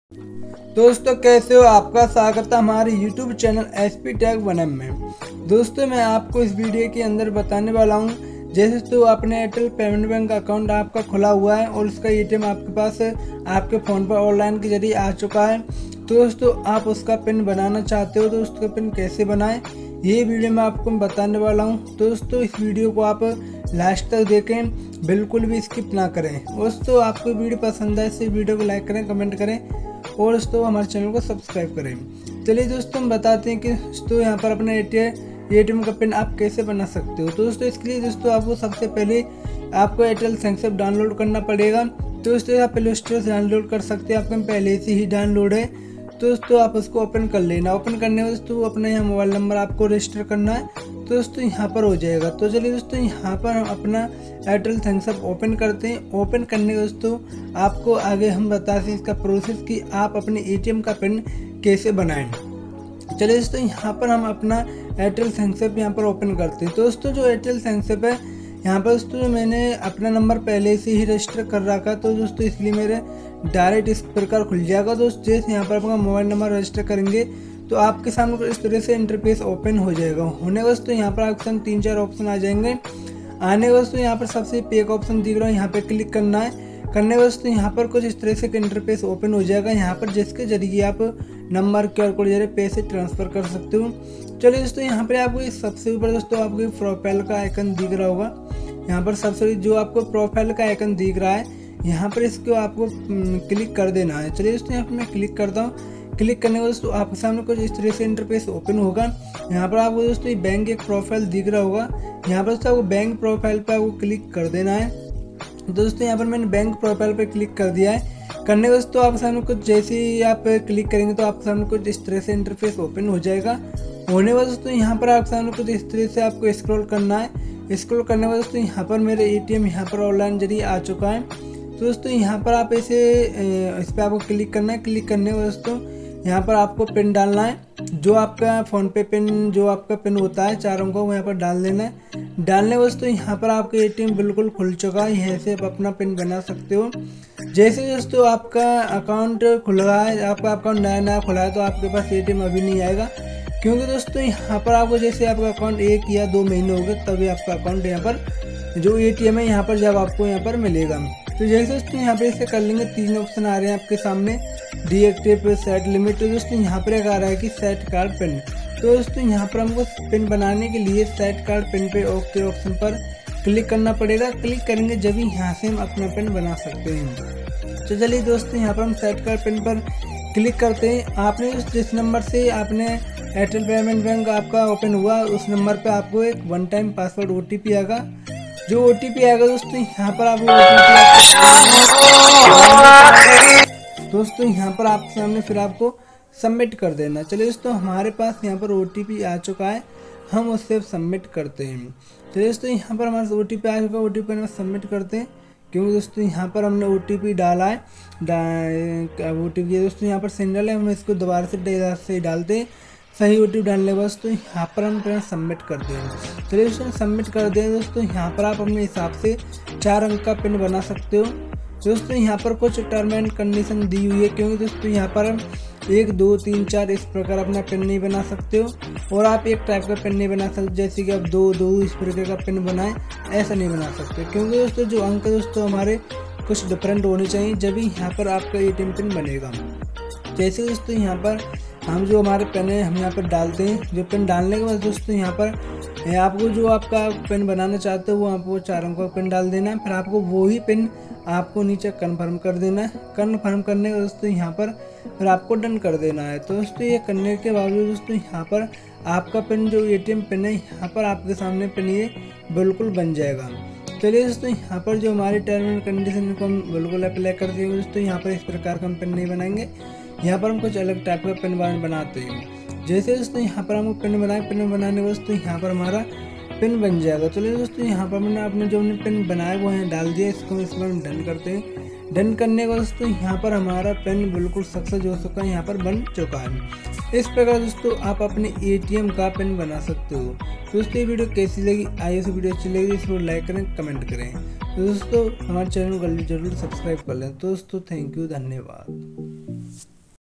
Generate Airtel Payment Bank ATM Pin | Airtel payment Bank ka ATM Pin Generate करना सीखें
Hello Doston,
Mera Name Satish parjapati hai aur aap
dekh rahe rahen hain aapka apne YouTube channel ``SP Tech 1M ''

आपका एयरटेल में अकाउंट खुला हुआ है और एटीएम का पिन जनरेट करना चाहते हो तो मैंने इस वीडियो में यही बताया है कि आप एटीएम का पिन कैसे बनाएं | यह वीडियो आपको पसंद आए तो इस वीडियो को लाइक करें कमेंट करें और हमारे चैनल को सब्सक्राइब करें | धन्यवाद